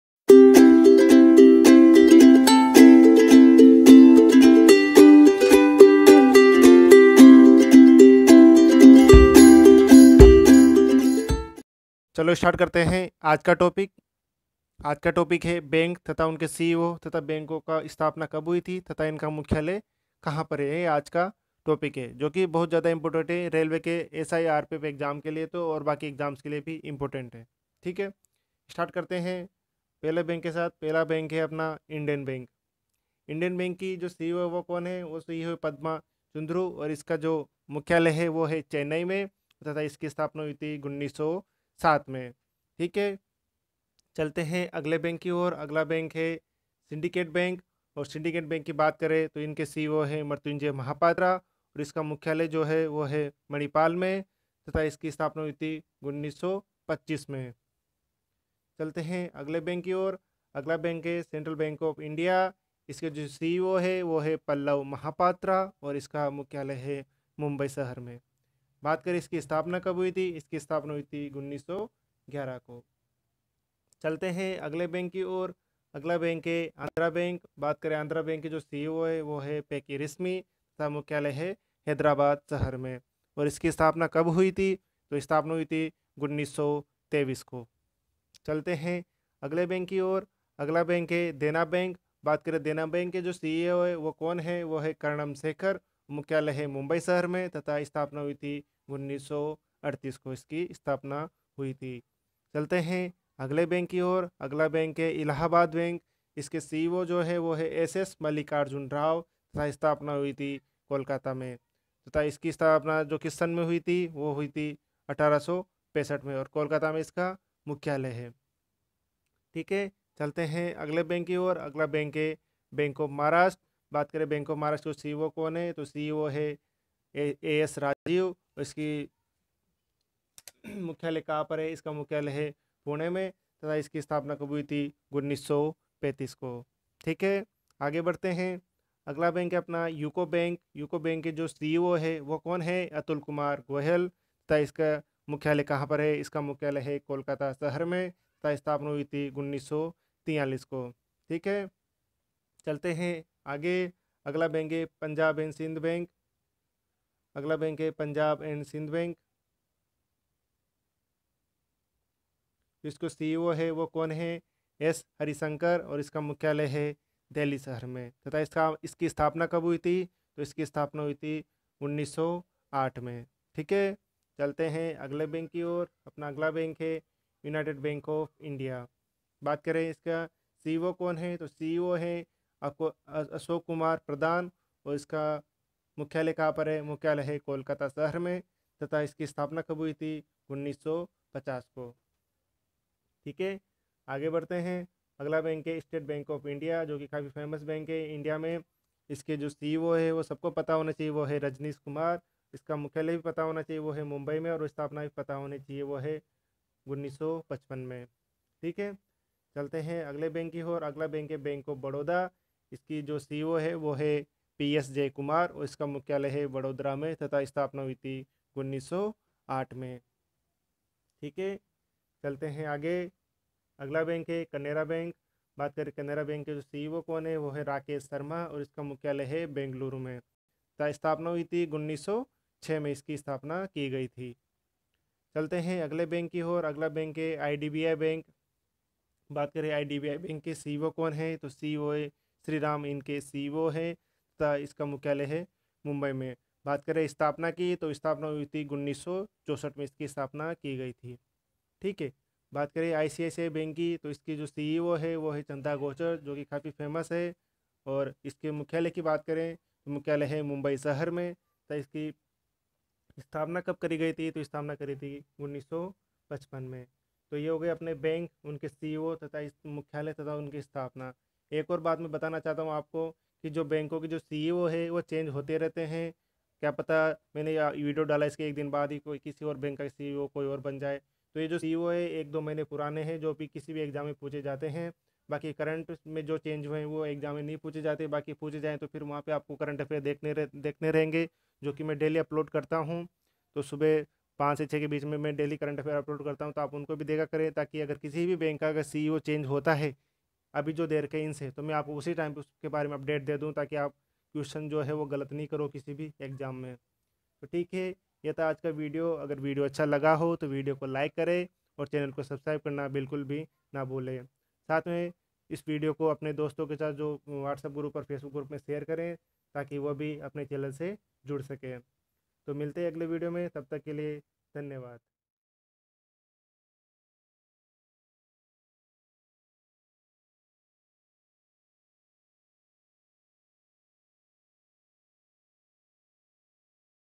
Banks & their CEO & headquarters & starting year, important for RPF SI & constable& SSC & bank exam.
In this video I covered all important banks and their CEO including their headquarters and the of starting in sequence to understand each bank details.